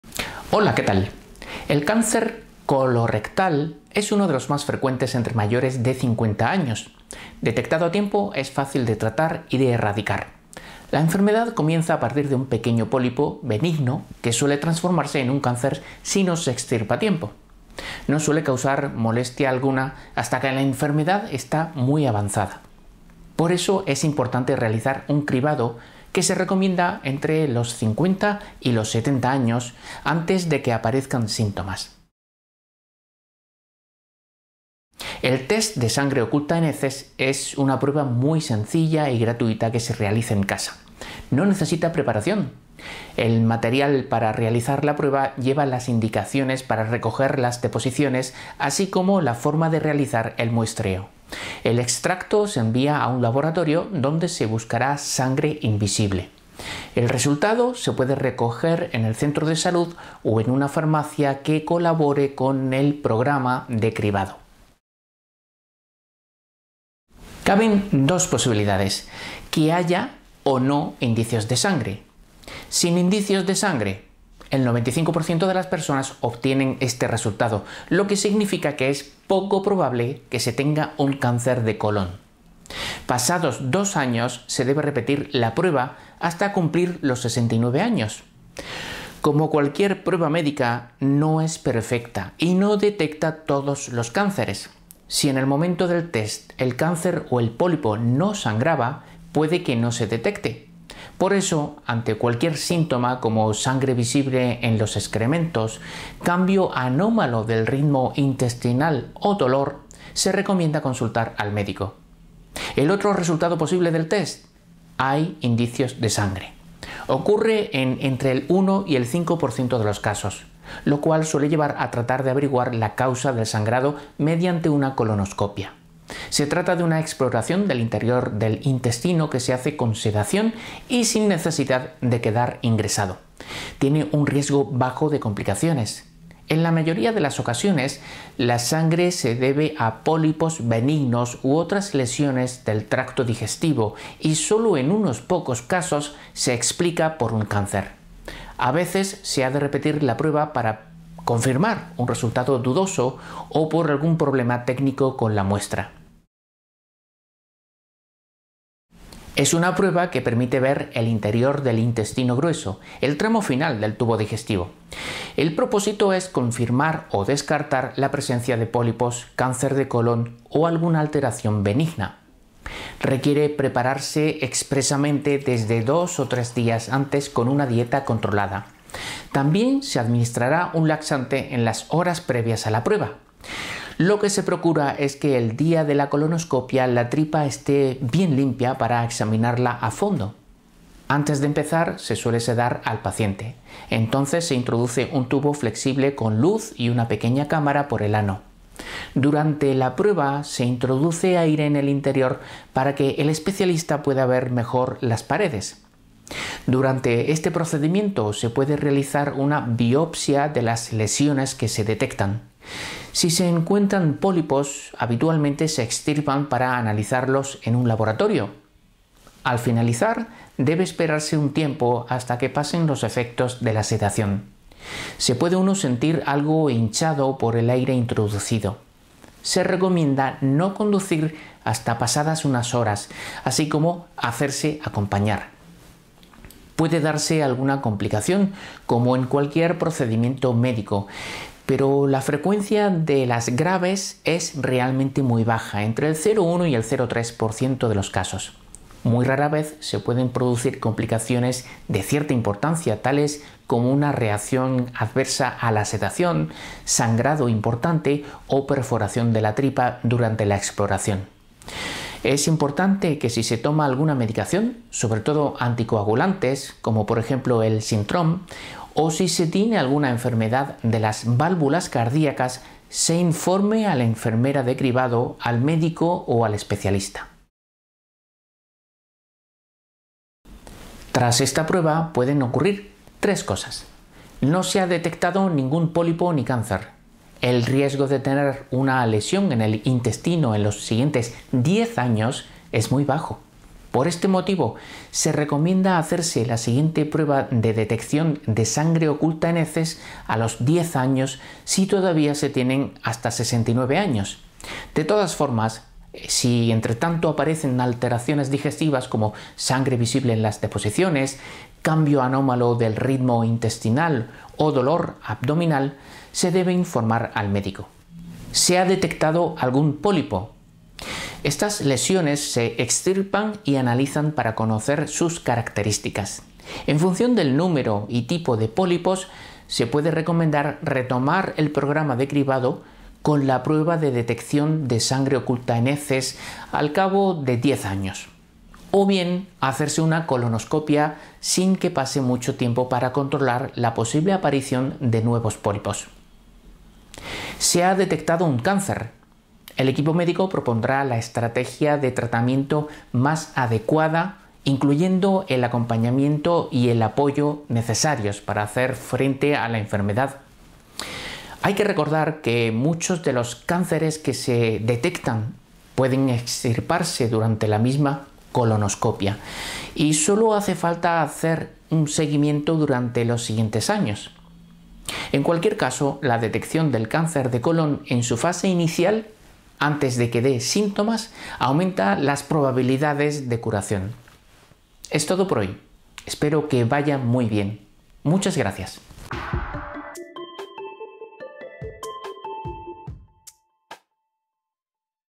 Prevenir el cáncer de colon
El cáncer colorrectal es uno de los más frecuentes entre mayores de 50 años. Detectado a tiempo, es fácil de tratar y de erradicar. La enfermedad comienza a partir de un pequeño pólipo benigno que puede transformarse en un cáncer si no se extirpa a tiempo. No suele causar molestia alguna hasta que la enfermedad está muy avanzada. Por eso es importante realizar un cribado, que se recomienda entre los 50 y los 70 años, antes de que aparezcan síntomas.

0:00 Introducción
0:33 La prueba de sangre oculta en heces
1:06 Resultados de la prueba
2:37 La colonoscopia
4:38 Los resultados de la colonoscopia